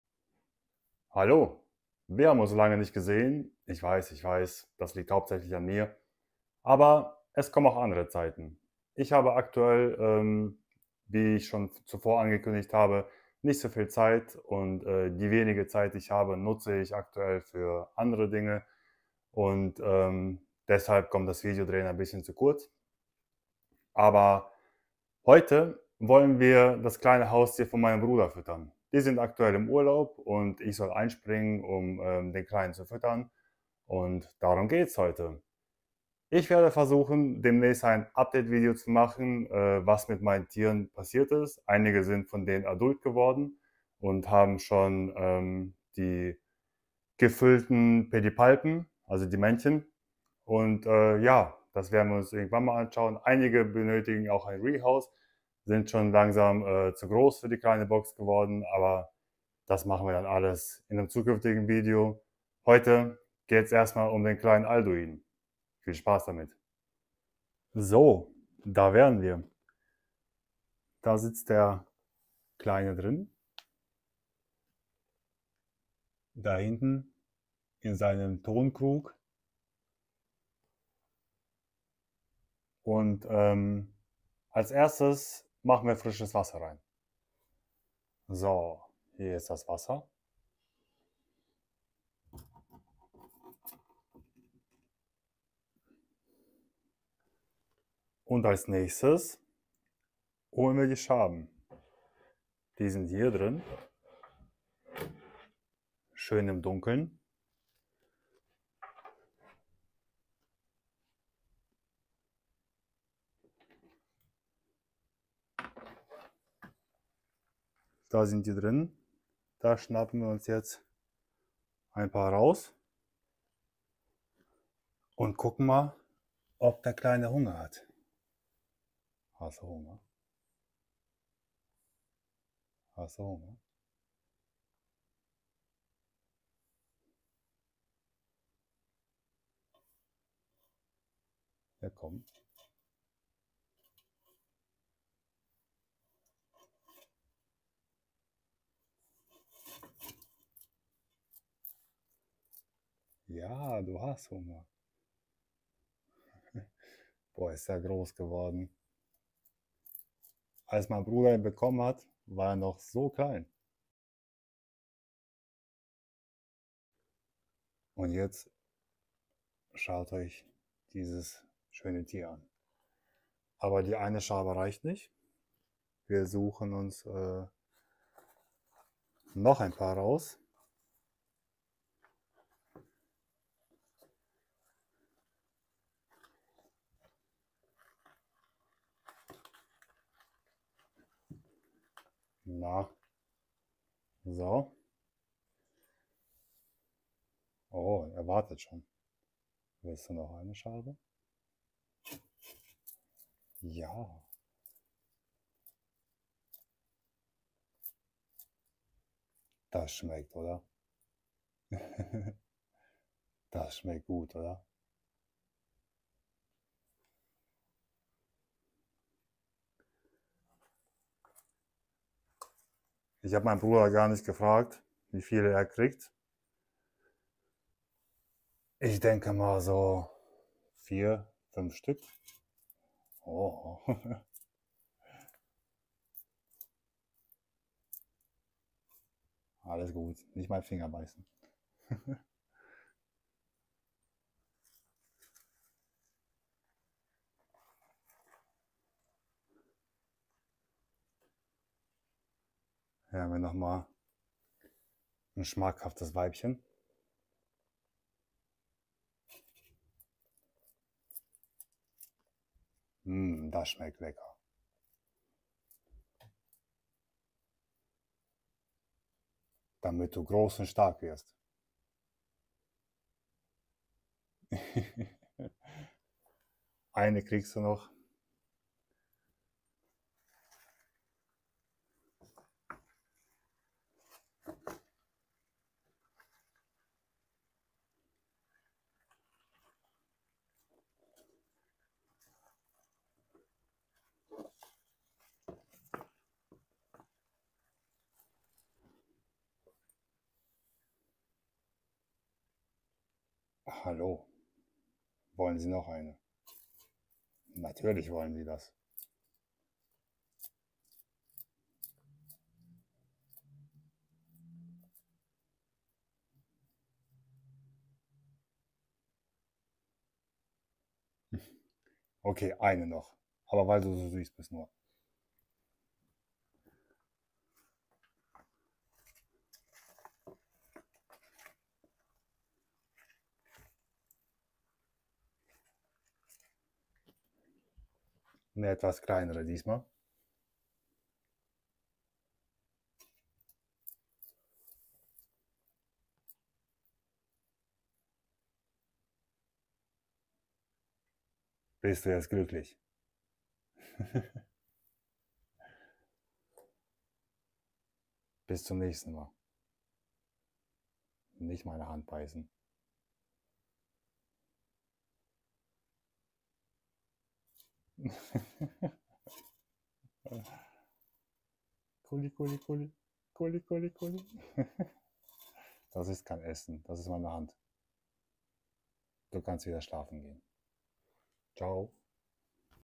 Bartagame füttern 😋🥰
Meine Familie ist im Urlaub und ich füttere das Haustier meines Bruders 🥰 Wahnsinn wie groß er geworden ist!